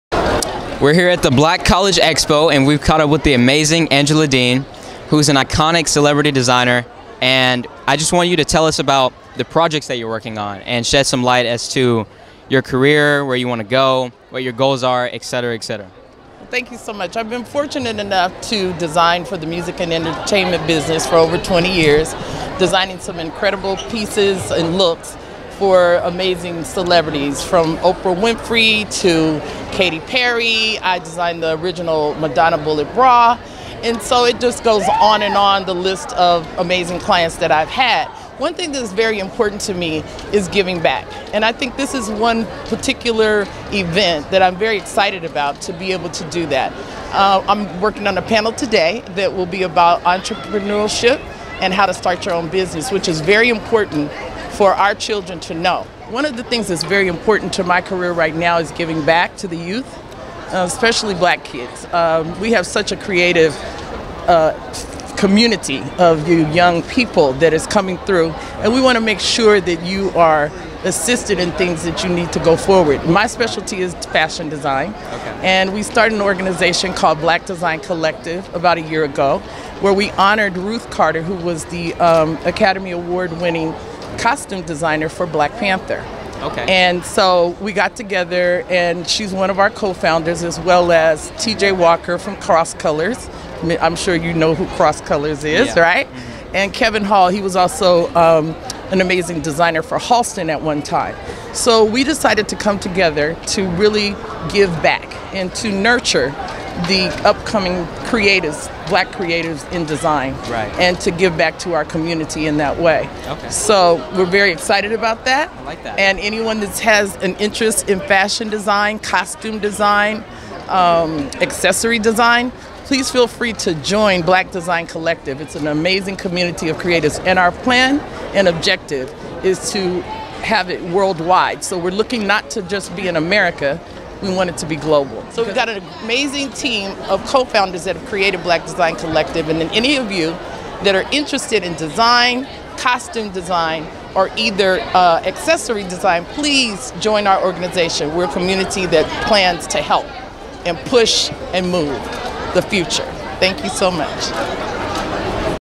Celebrity designer Angela Dean talks giving back and new platform with Oscar Winner Ruth Carter
Celebrity fashion designer Angela Dean who has worked with Madonna, Katy Perry, Oprah, and many others stops by to chat with PopSource TV's Eshun Melvin about her new platform "Black Design Collective" that she formed with Academy Award Winning Costume Designer for Black Panther Ruth Carter, former Halston designer Kevin Hall and Cross Colors designer TJ Walker.